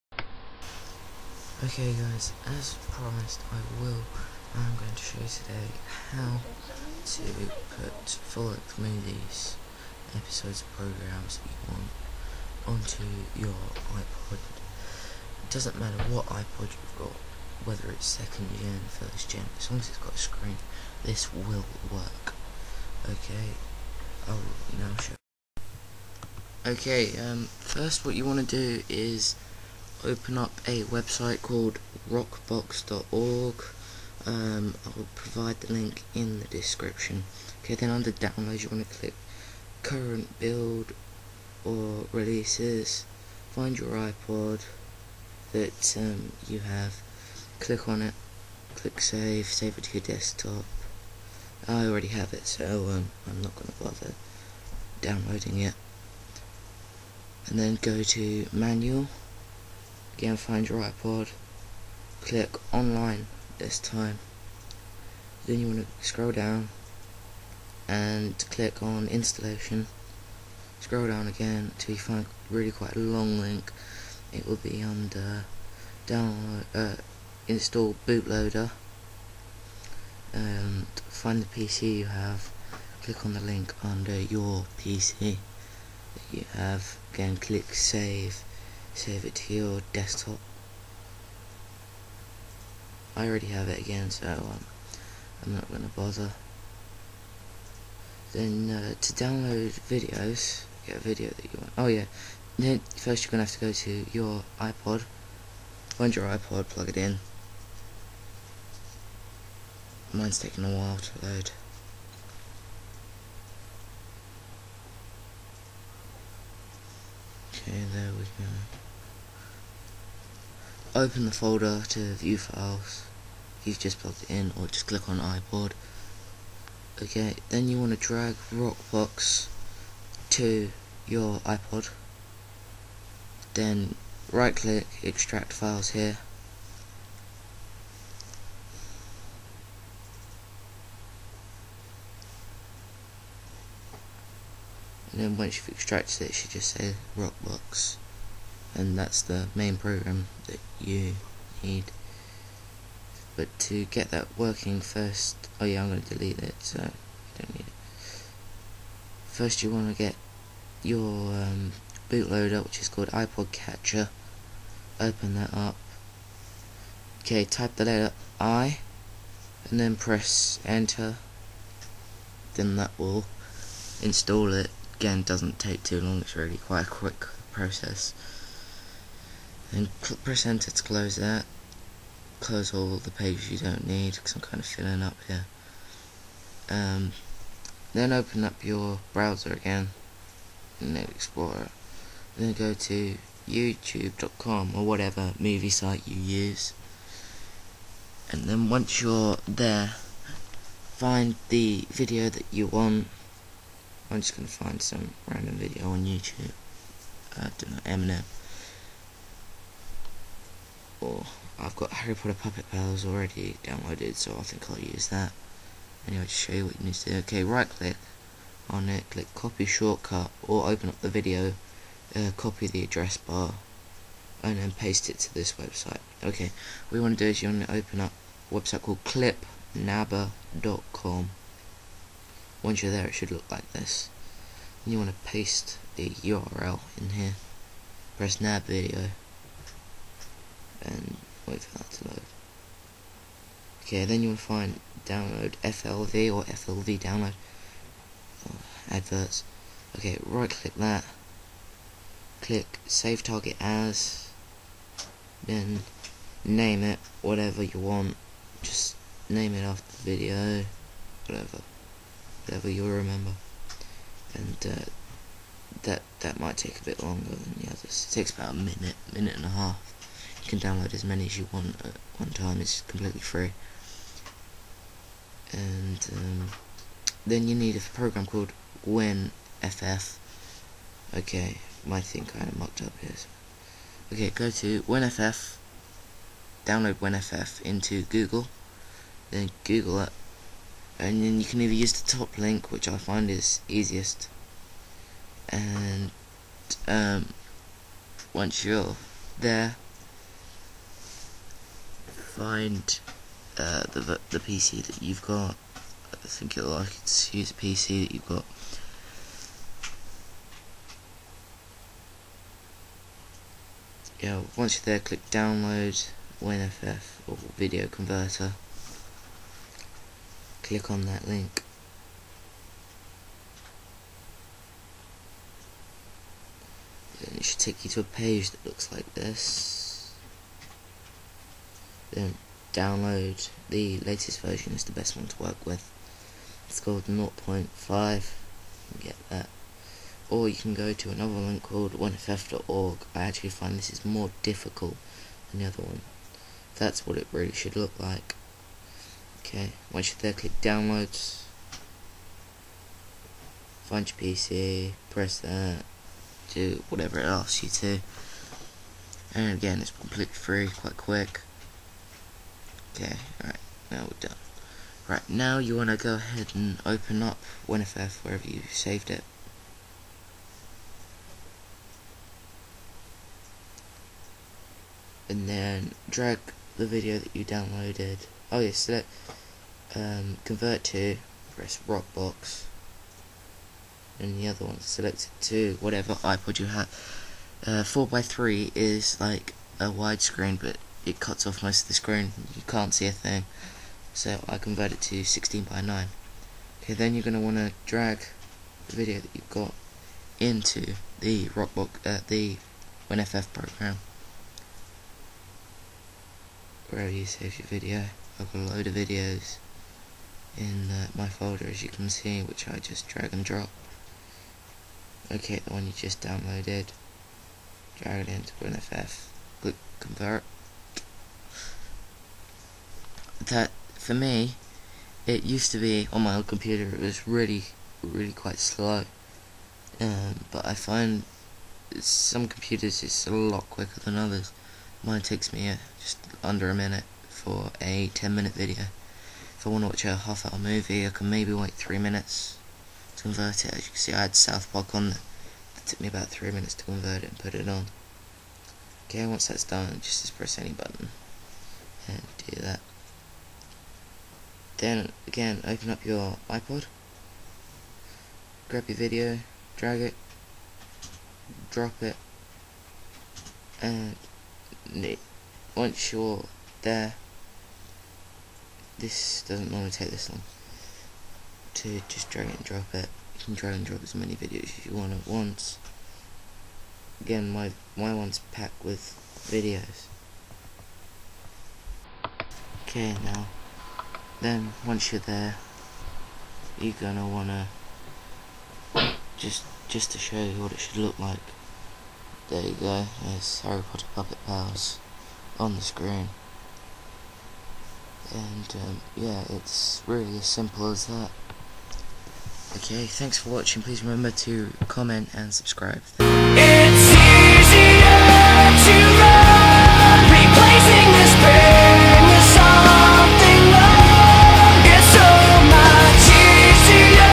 how to put videos on a ipod nano
in this video i will walk you through step by step how to put your clips or full length movies directly to your ipod nano or whatever ipod you have this programme also works with other mp3 players and is completely free and legal